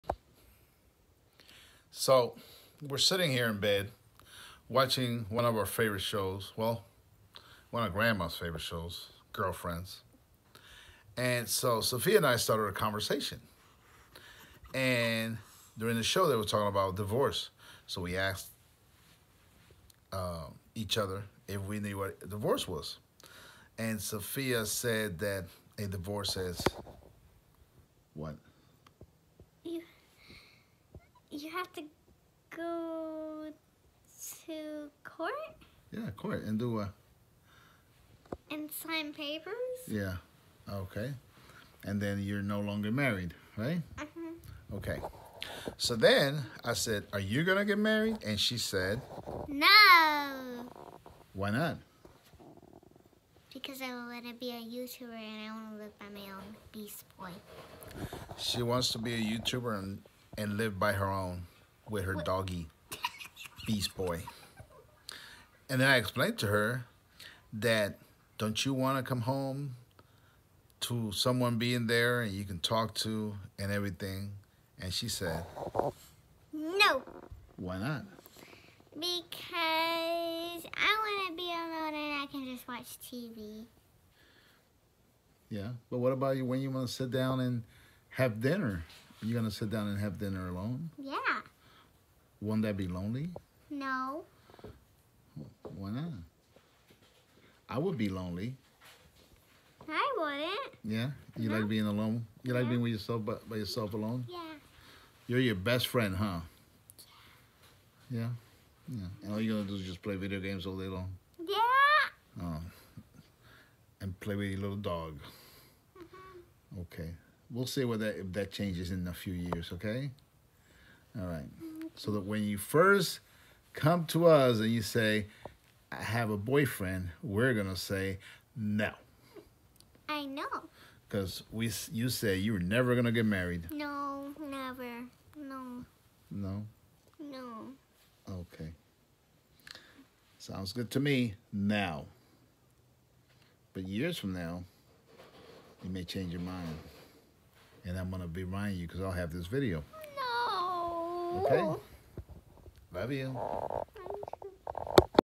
Sofia tells grandpa she’s never getting married
Sofia says she will never get married